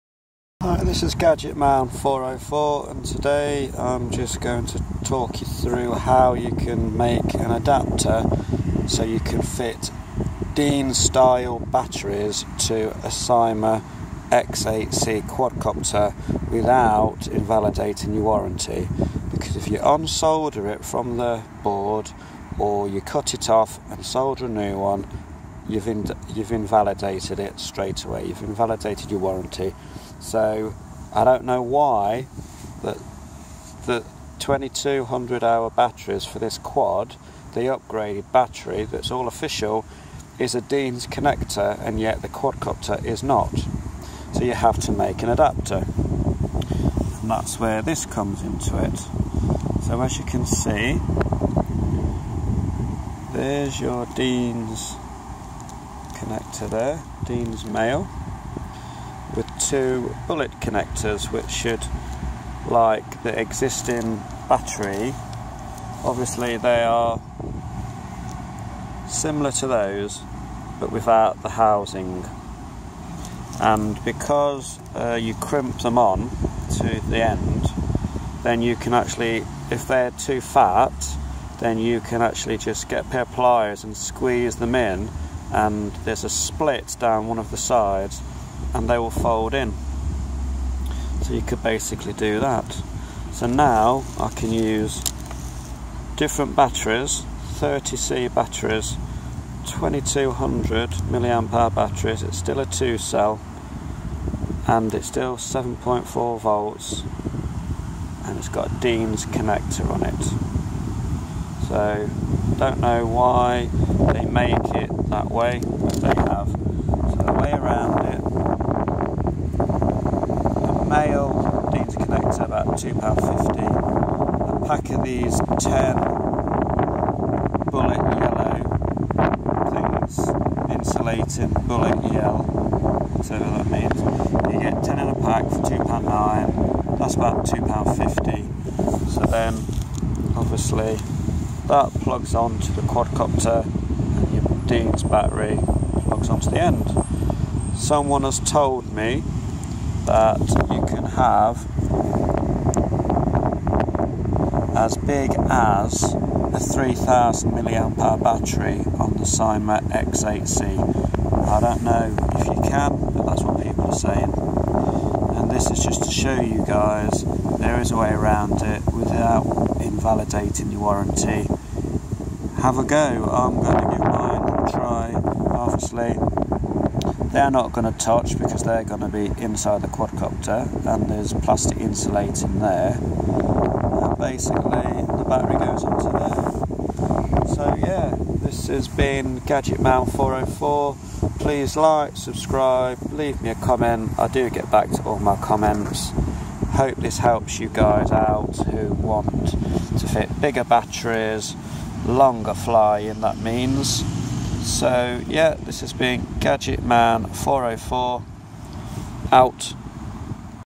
Battery adaptor mod Syma x8c 4K
Hi guys just a update on how can use bigger battery's on syma x8c one problem ive found.Is that the upgrade battery 2200 mah battery is a dean's connector but the quadcopter is not so here how to make an adaptor so you can original battery plus bigger ones with diffrent connectors without invalidating your warrenty enjoy thanks for watching please  subscribe like comment leave a message i get back to all messages left me gadgetman 404 out